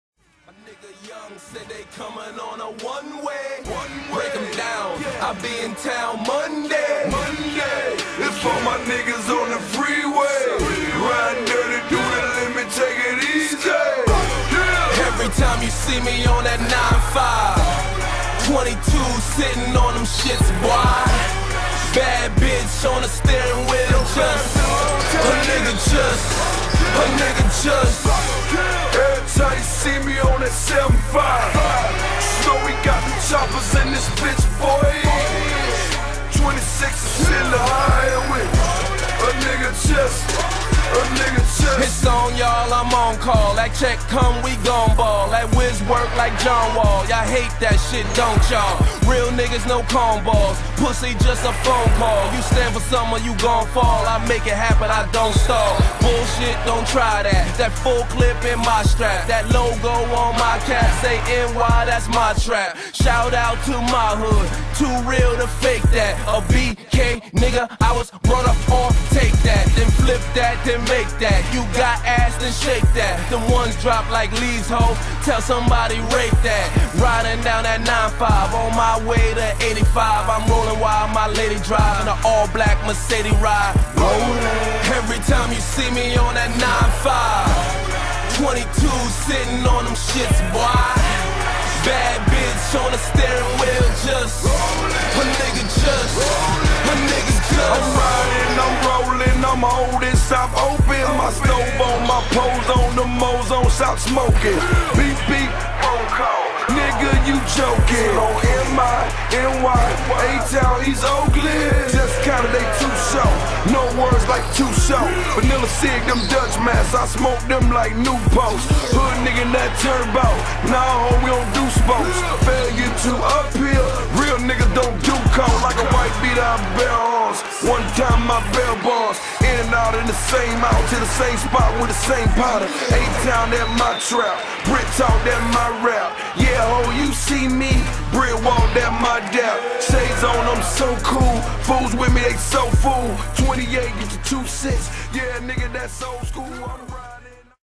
Julian BasketBall Mixtape
Shooting Guard plays with dedication, heart, and soul when team is down he will do what ever he can to get the WIN!! Check it out and Comment!!!